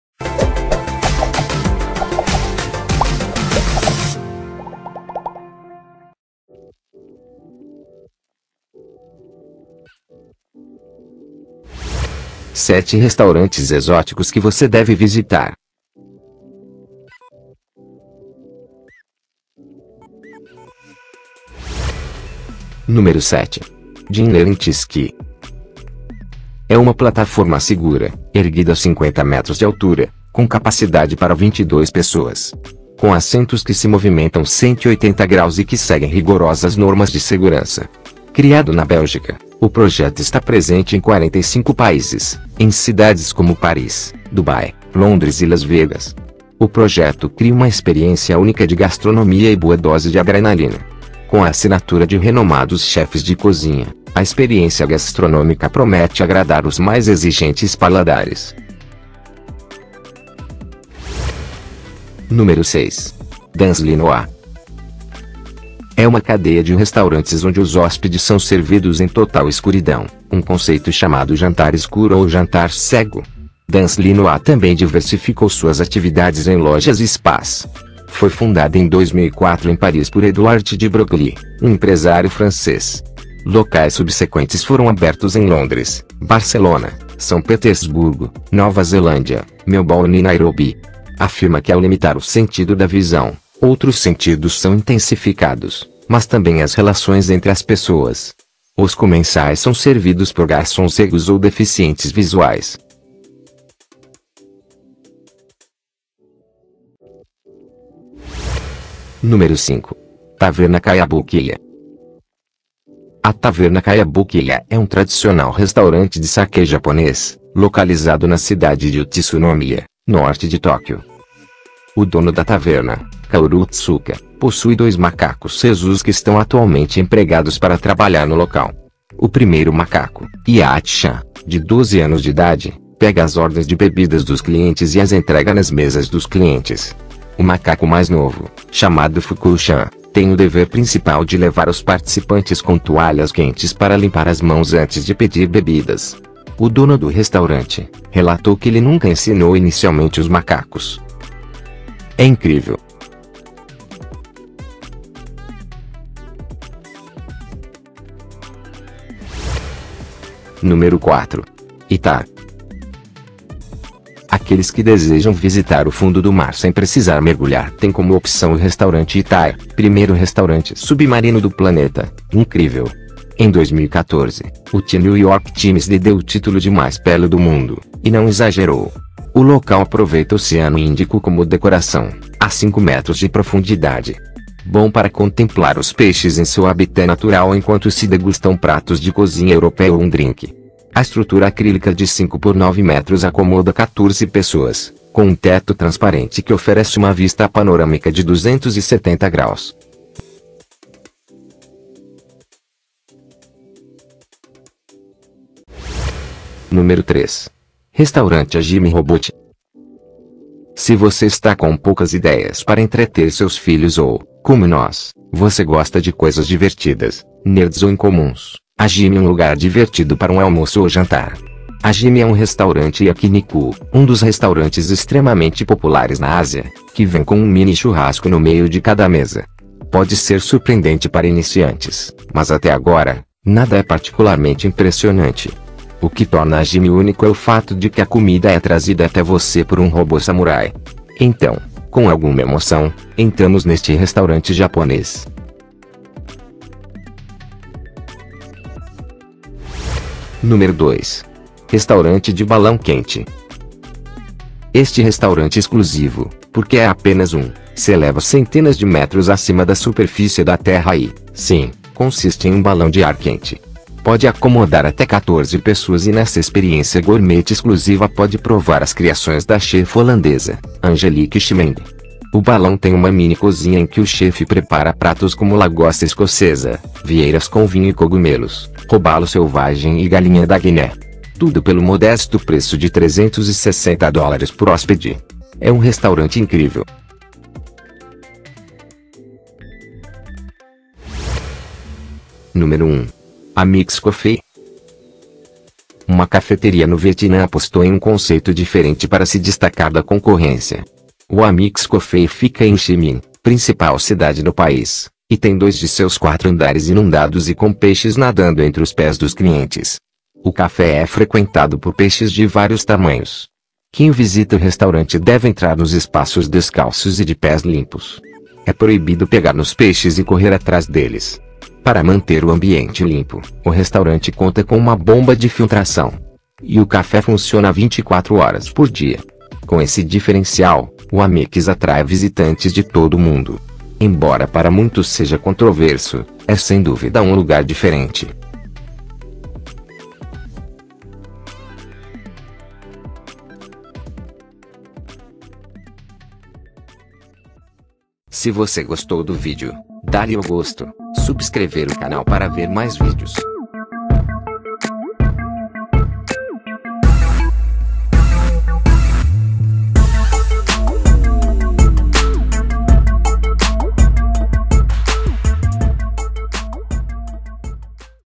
7 Restaurantes Exóticos Que Você Deve Visitar
Top 7 restaurantes mais estranhos e incríveis que você deve visitar.